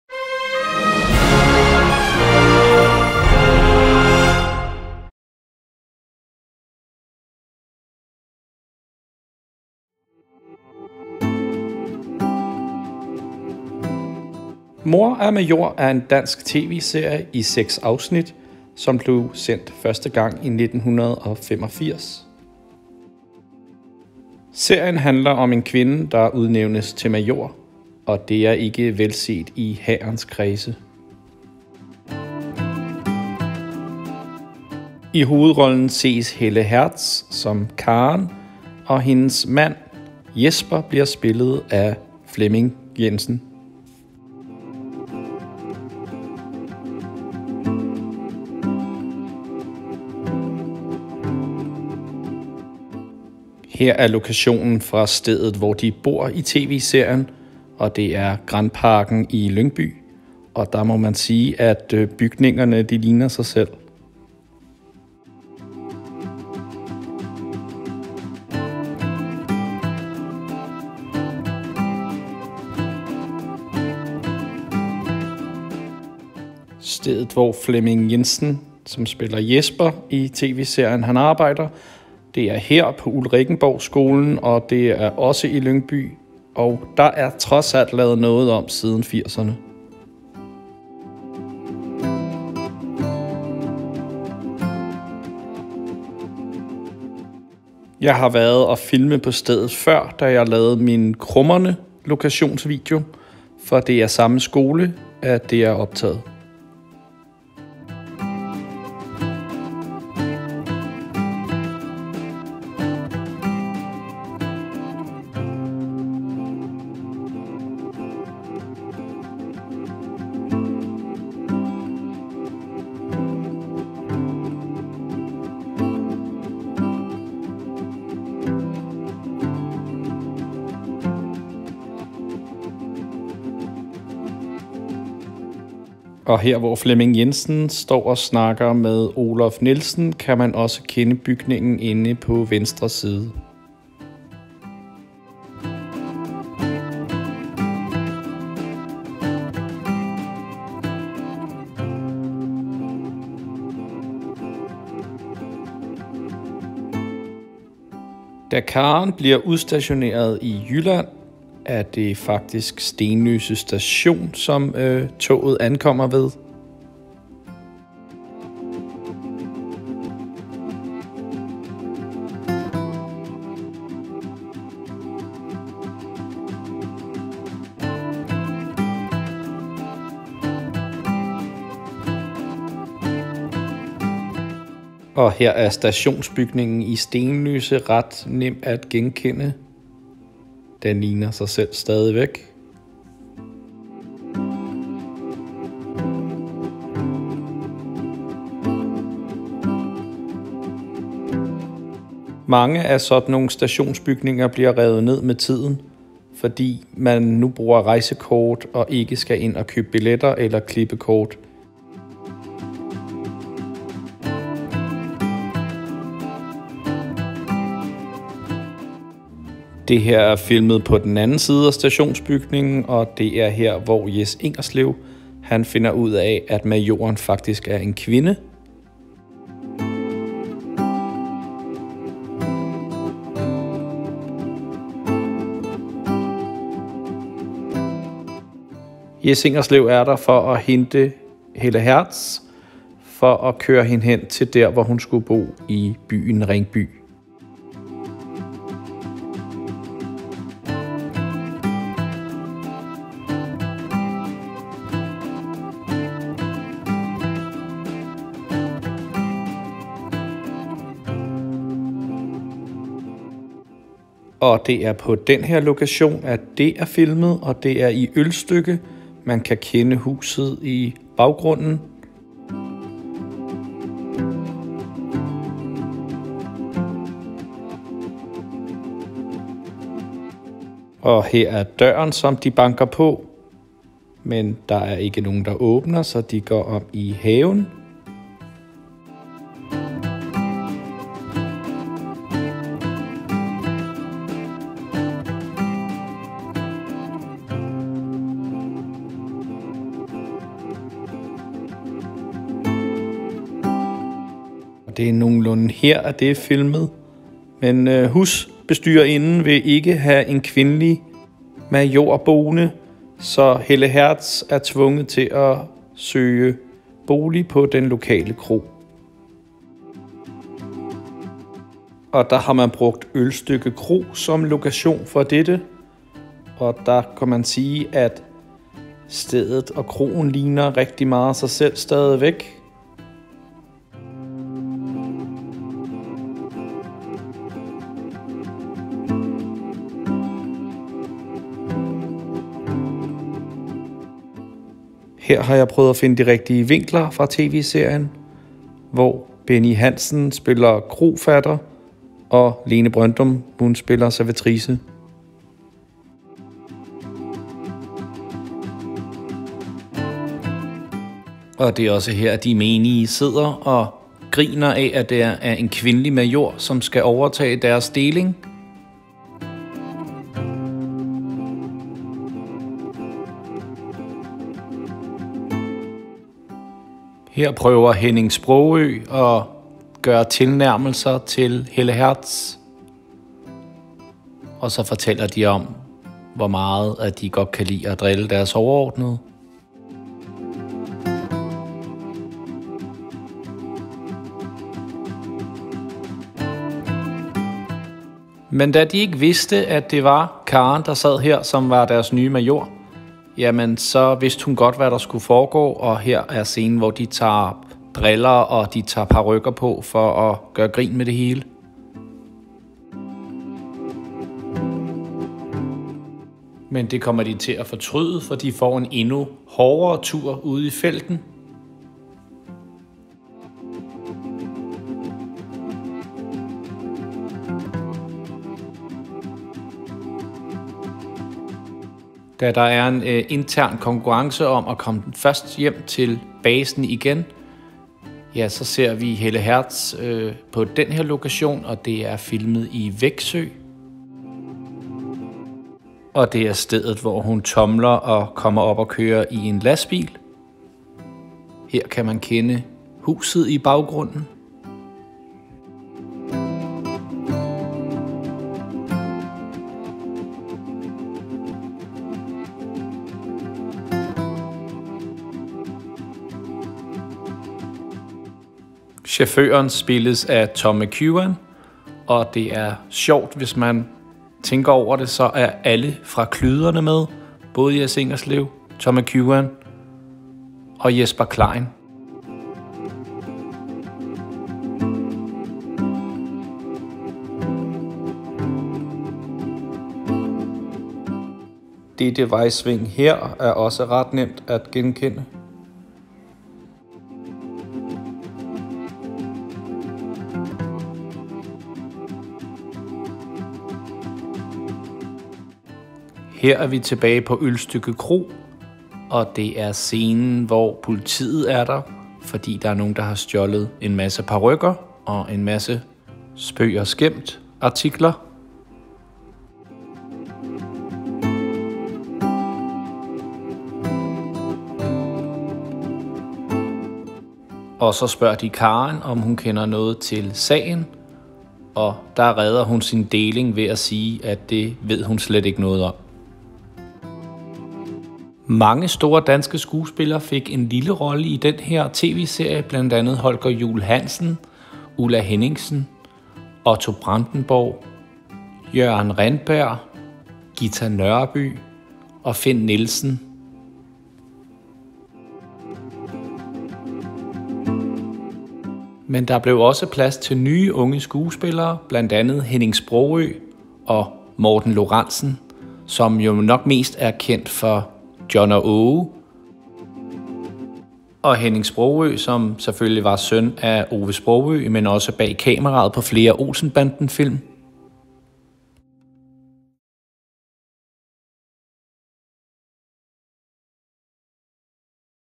Filmlokationer - Mor er Major - Dansk serie Danmarks Radio 1985
Serien fra Danmarks Radio fra 1985 med Helle Hertz og Flemming Jensen.
Her har jeg prøvet at finde nogen af stederne fra Serien .